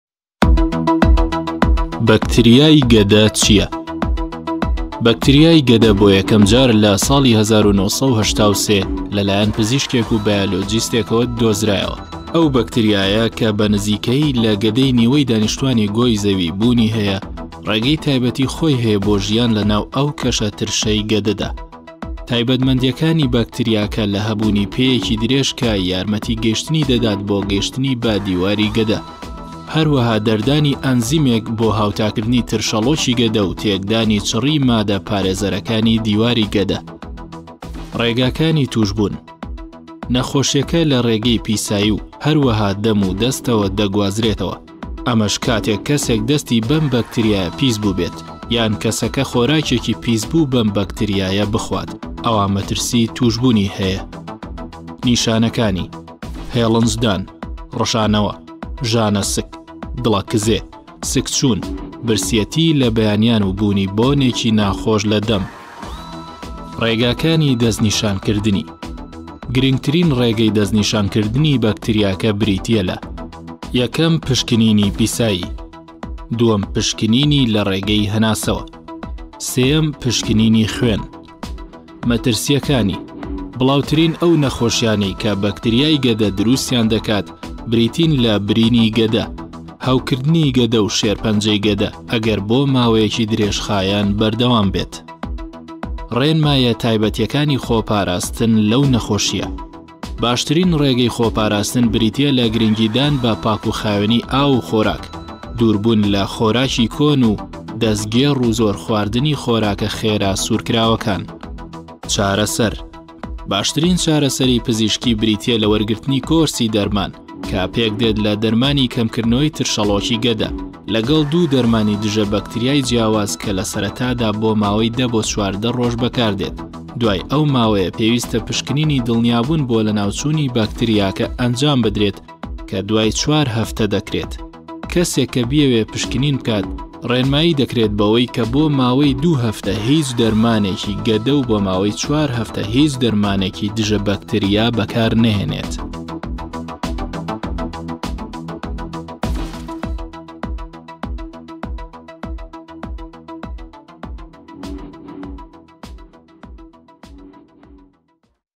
به‌كتریای گه‌ده‌ چیه‌؟ چۆن ده‌گوازرێته‌وه‌؟ نیشانه‌كانی تووشبوون پێیه‌وه‌ چیه‌؟
دیجیتاڵ_میدیای_په‌یام